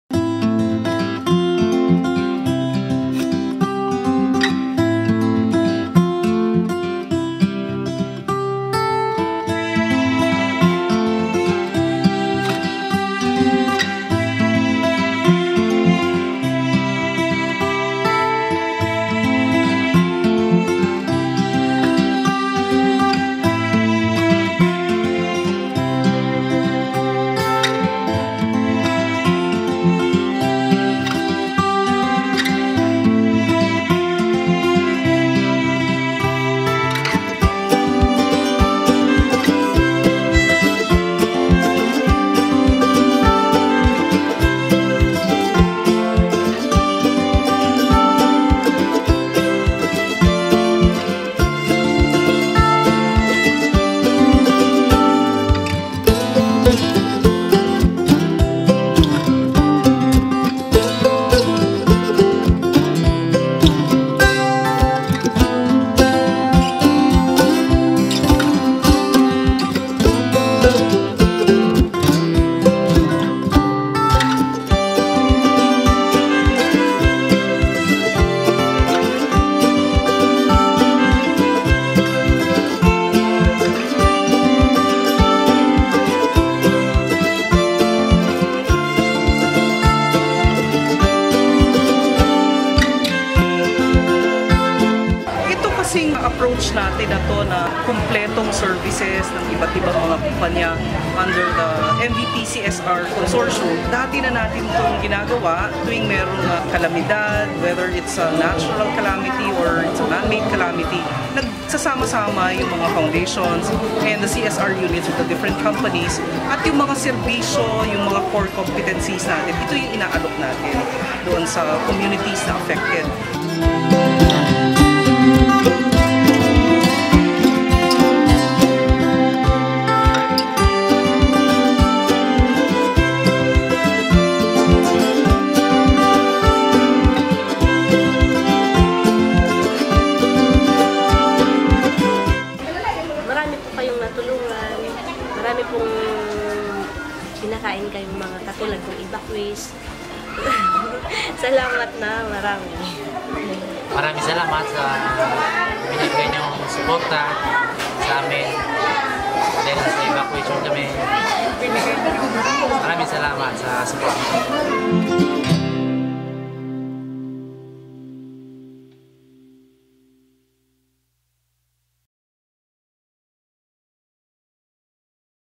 Smart joins Tulong Kapatid operation in Albay
Smart Communications, joined the TulongKapatid operations to provide assistance to Albay families affected by the Mayon eruption.